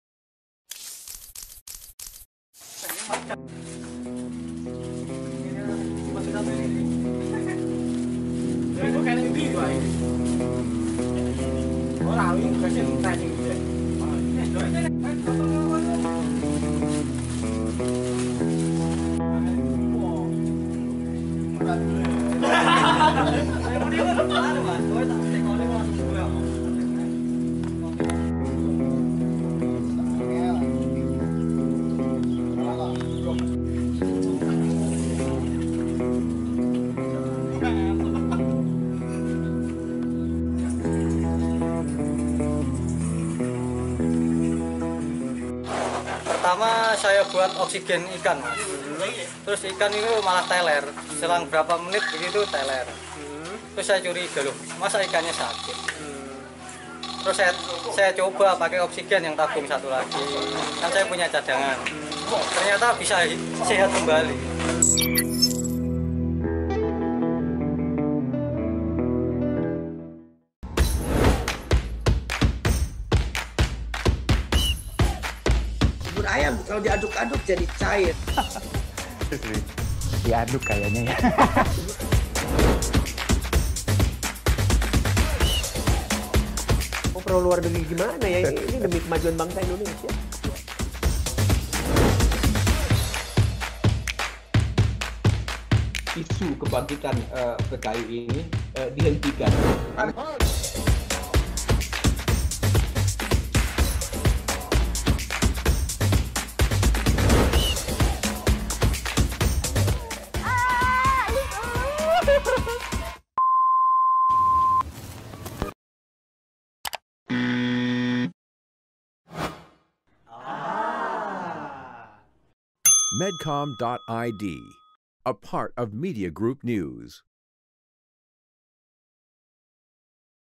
Tabung Isi Oksigen Palsu Resahkan Pedagang Ikan di Tulungagung
Temuan tabung oksigen yang diduga berisi oksigen palsu di Tulungagung, Jawa Timur meresahkan peternak dan pedagang ikan hias. Kasus dugaan oksigen palsu kini tengah dalam penyelidikan kepolisian.

Dugaan beredarnya oksigen palsu di kalangan peternak dan pedagang ikan hias di Tulungagung, Jawa Timur ditemukan oleh komunitas pedagang ikan hias di Desa Bendiljatiwetan, Kecamatan Sumbergempol.

Kasus temuan dugaan oksigen palsu tersebut kini tengah dalam penyelidikan pihak kepolisian. Tabung yang diduga berisi oksigen palsu dan ikan yang mati dibawa sebagai barang bukti.

Polisi berencana meminta bantuan ahli untuk memastikan jenis gas di dalam tabung tersebut. Polisi juga memintai keterangan sejumlah saksi untuk mengetahui asal tabung gas yang di duga berisi oksigen palsu tersebut. Dwi Wianto/Metro TV.